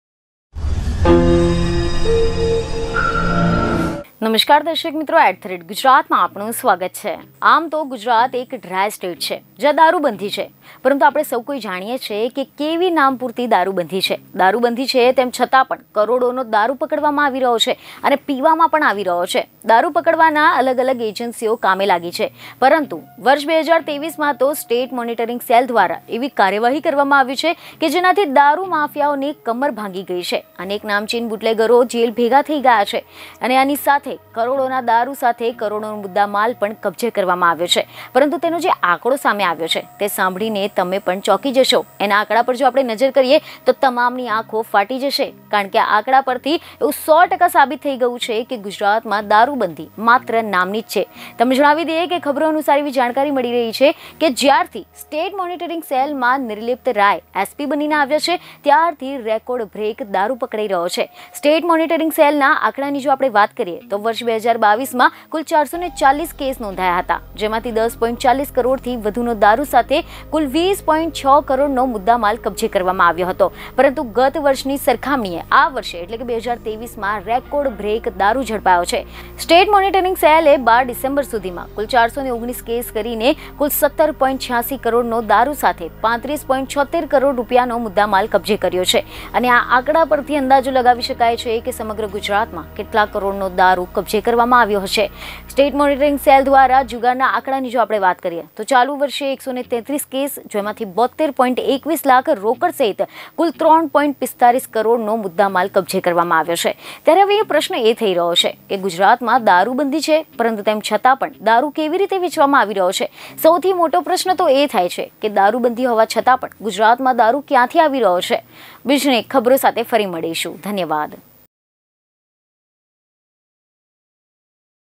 દારૂબંધી છે એવાં ગુજરાતમાં સ્ટેટ મોનિટરીંગ સેલે કેટલો દારૂ પકડ્યો જાણો છો? | Liquor Ban In Gujarat
સ્ટેટ મોનિટરિંગ સેલમાં નિર્લિપ્ત રાય SP બનીને આવ્યા ત્યારથી રેકોર્ડ બ્રેક દારૂ પકડાઈ રહ્યો છે. સ્ટેટ મોનિટરિંગ સેલના આંકડાની વાત કરીએ તો વર્ષ 2022માં કુલ 440 કેસ કર્યા હતા, જેમાંથી 10.40 કરોડથી વધુનો દારૂ સાથે કુલ 20.06 કરોડનો મુદ્દામાલ કબ્જે કરવામાં આવ્યો છે.